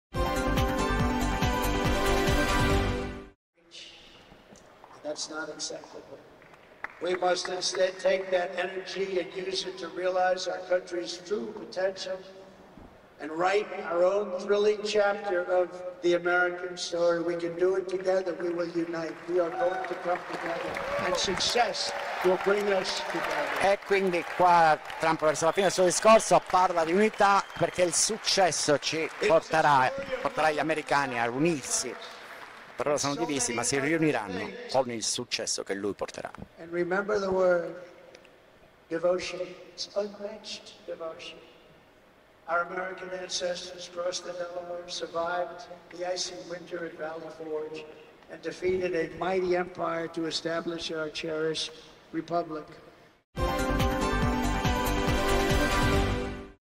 Trump "Col mio successo unisco l'America"
MILWAUKEE (STATI UNITI) (ITALPRESS) - "Col mio successo unirò l'America". Lo ha detto Donald Trump alla convention dei Repubblicani parlando dell'importanza dell'unità.
mgg/gtr (video di Stefano Vaccara)